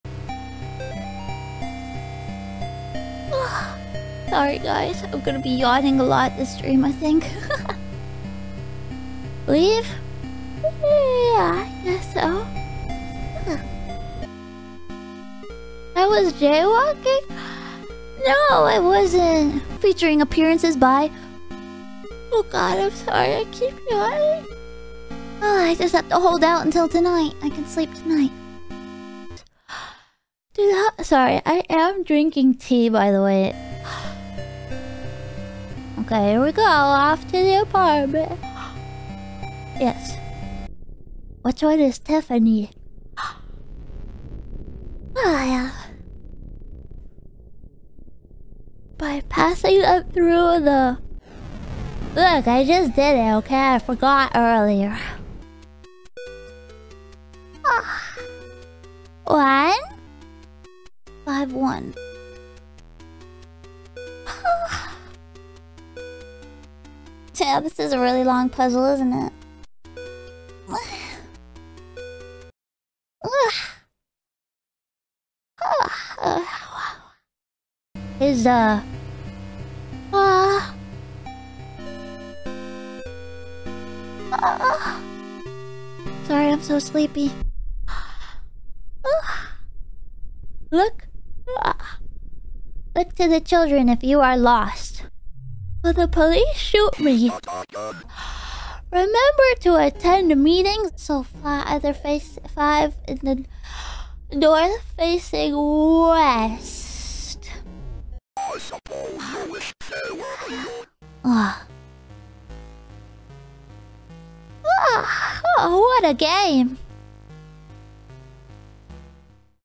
Sleepy Pomu Yawning Compilation【Nijisanji EN】
Yawns so cute TSKR. Hopefully she gets a nice rest though